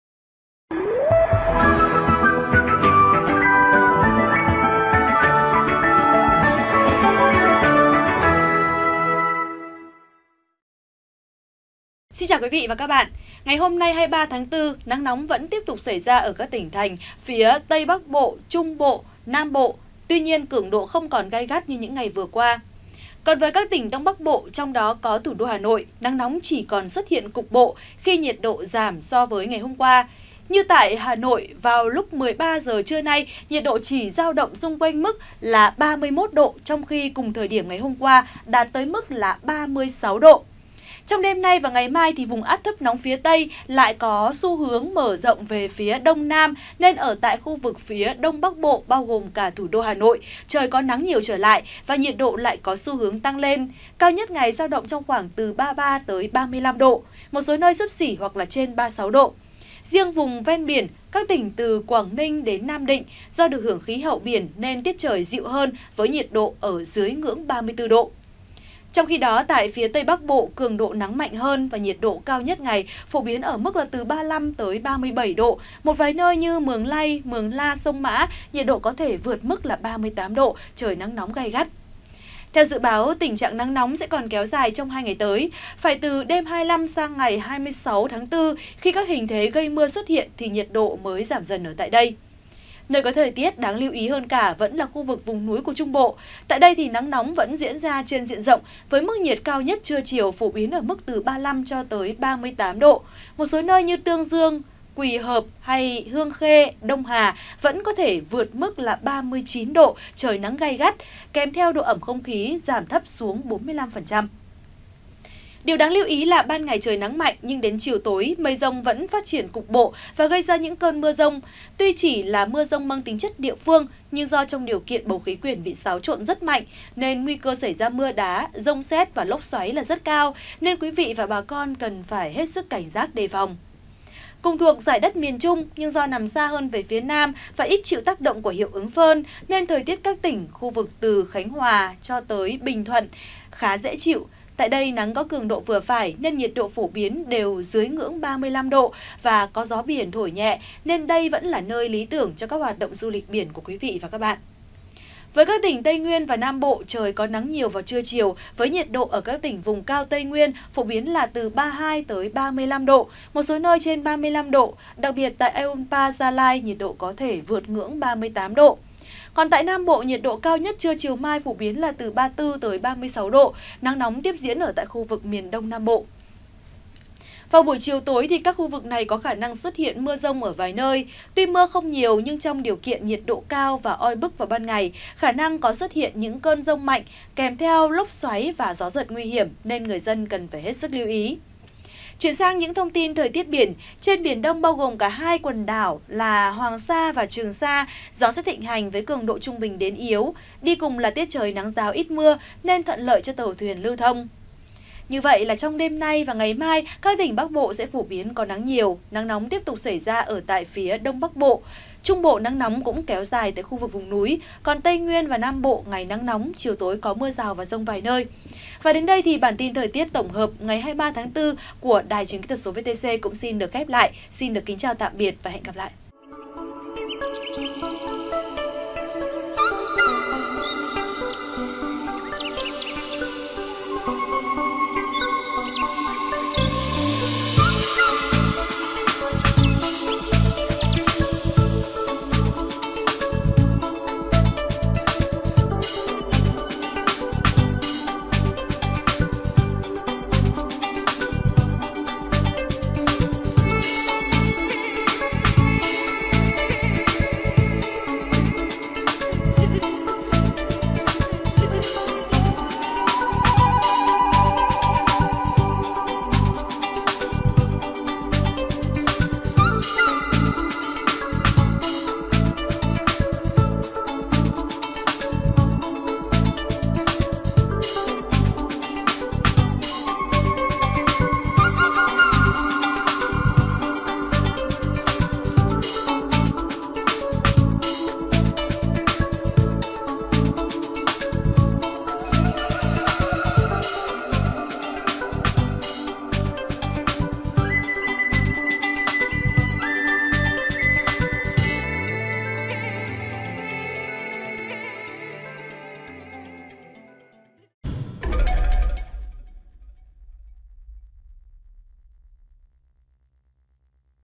Thời tiết tổng hợp 23/04/2019: Chưa có biến động lớn về thời tiết trên cả 3 miền | VTC14
VTC14 | Trong đêm nay và ngày mai, các tỉnh Bắc Bộ sẽ phổ biến có nắng nhiều, nắng nóng tiếp tục xảy ra ở phía Đông Bắc Bộ. Trung Bộ nắng nóng cũng kéo dài tại khu vực vùng núi. Còn Tây Nguyên và Nam Bộ ngày nắng nóng, chiều tối có mưa rào và dông vài nơi.